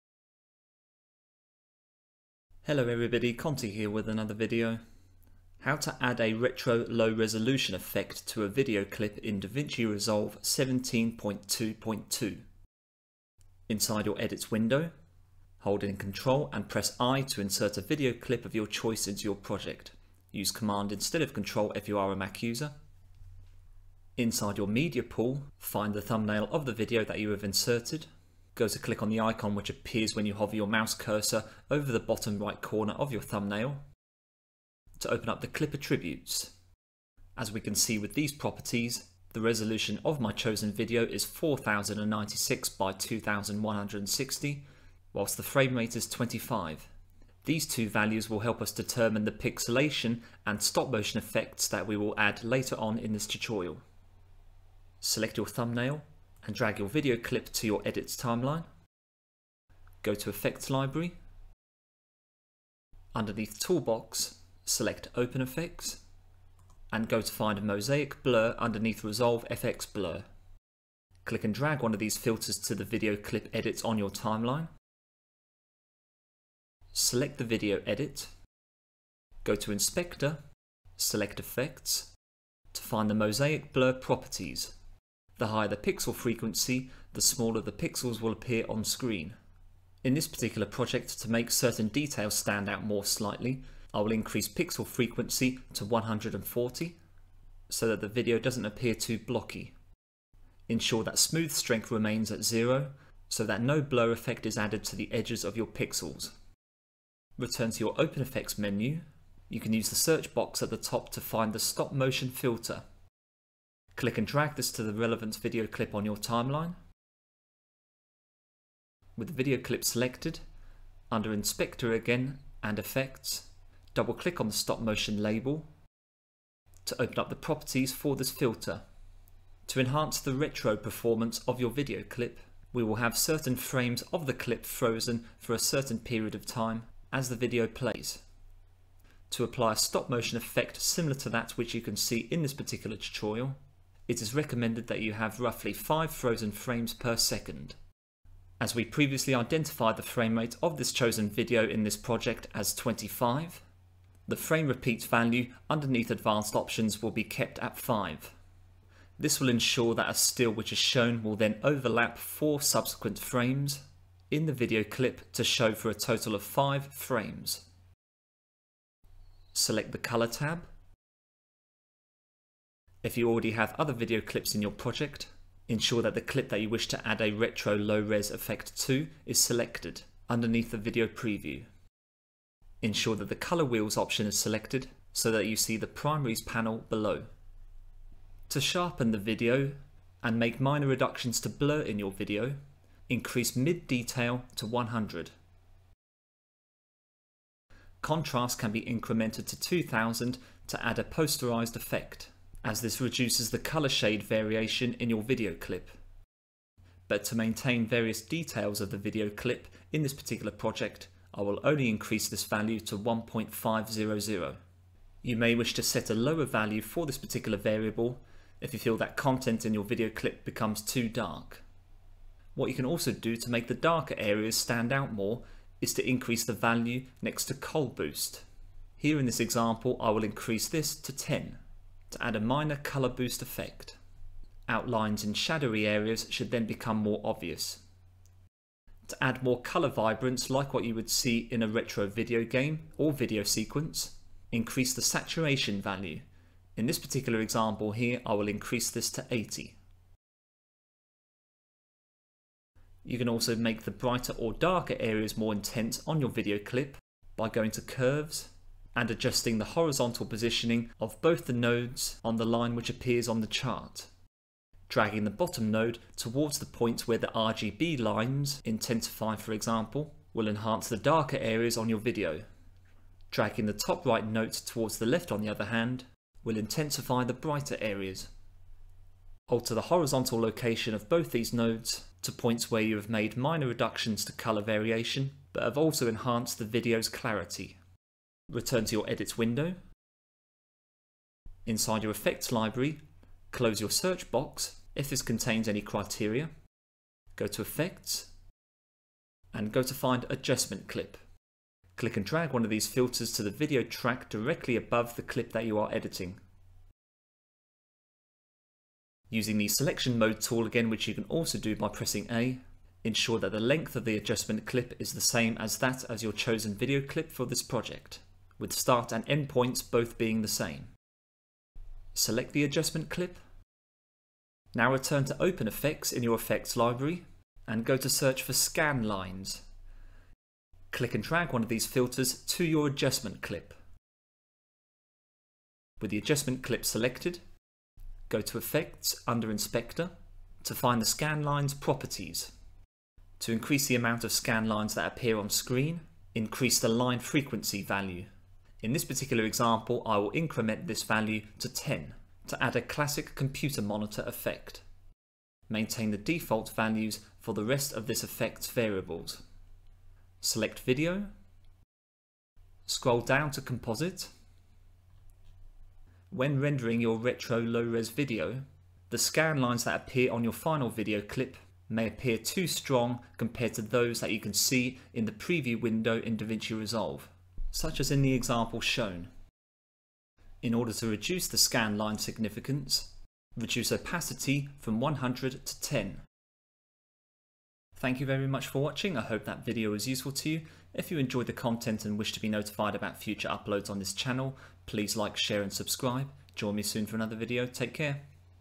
DaVinci Resolve Tutorial: How To Create a Retro Low Resolution Effect
DaVinci Resolve Tutorial #121. This video illustrates how to create a pixelated video clip with a low colour depth to make up a classic appearance. DaVinci Resolve 17.2.2 is used in this tutorial.